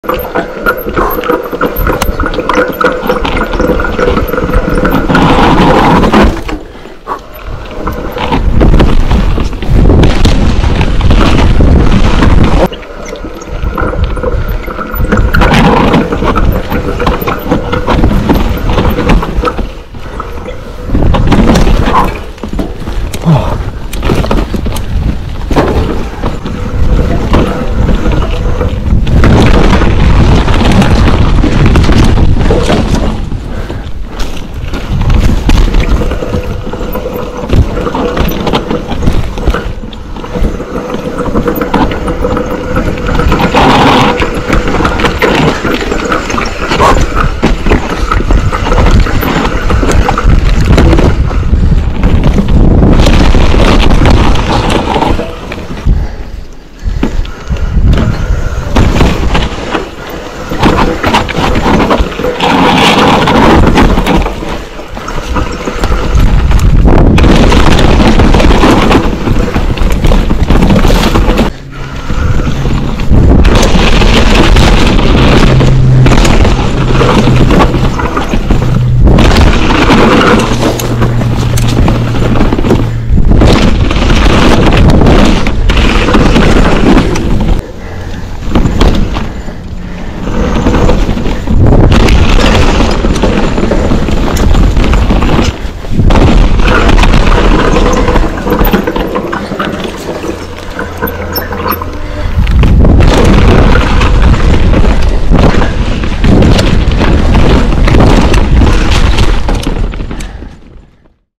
Spicy 🌶️ Double black Diamond MTB trail 🚴 Squamish, BC
Riding some challenging features and trail networks in the valleycliffe area of Squamish